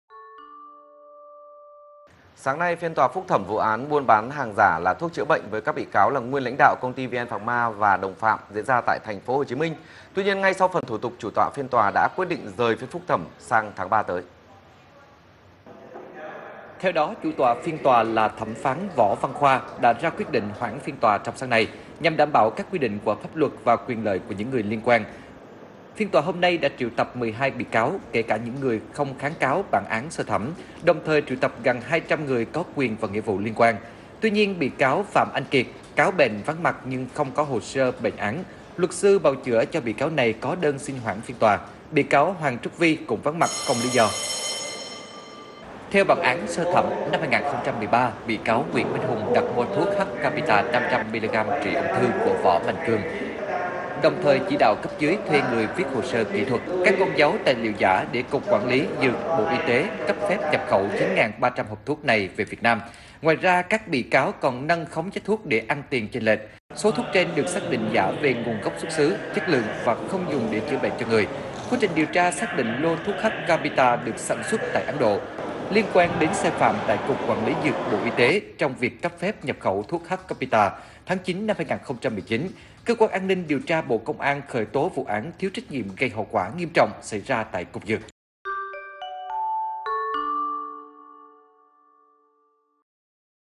Vì sao hoãn phiên phúc thẩm vụ VN Pharma? | VTC1
VTC1 | Theo kế hoạch, TAND cấp cao tại TPHCM mở phiên tòa xét xử phúc thẩm vụ buôn thuốc trị ung thư giả tại Công ty cổ phần VN Pharma. Tuy nhiên sau đó phiên tòa lại hoãn.